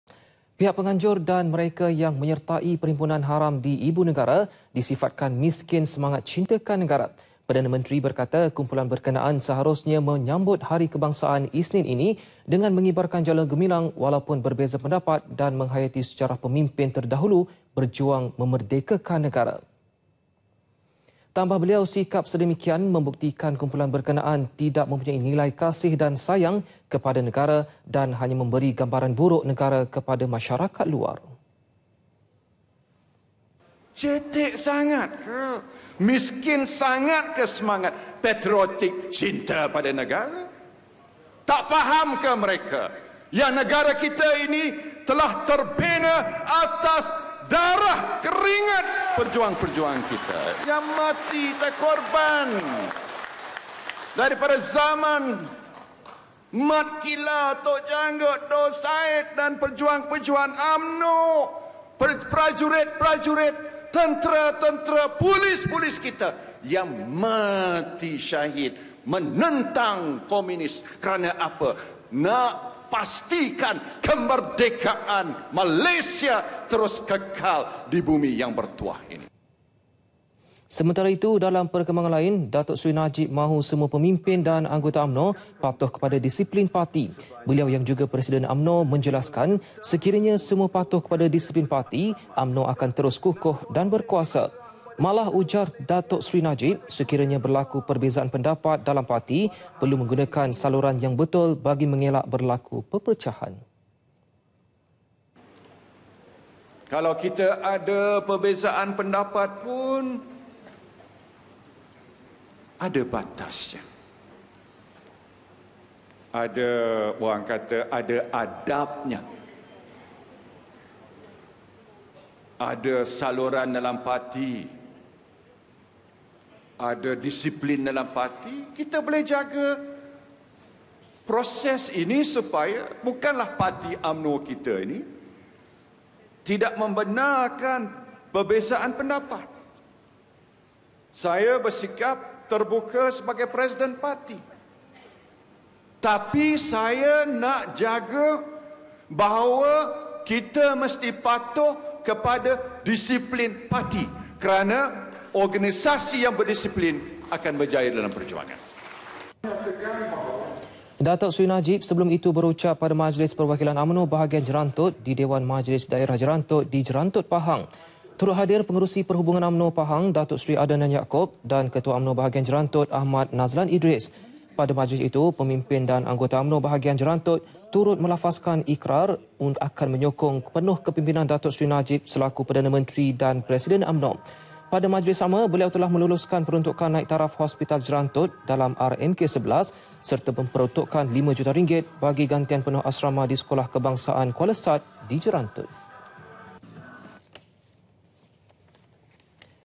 UMNO - PM: SERTAI PERHIMPUNAN MISKIN CINTAKAN NEGARA [29 OGOS 2015]
Pihak penganjur dan mereka yang menyertai perhimpunan haram di ibu negara, DISIFATKAN miskin semangat cintakan negara.

Perdana Menteri, berkata, kumpulan berkenaan seharusnya menyambut Hari Kebangsaan Isnin ini, dengan mengibar Jalur Gemilang walaupun berbeza pendapat dan menghayati sejarah pemimpin terdahulu berjuang memerdekakan negara.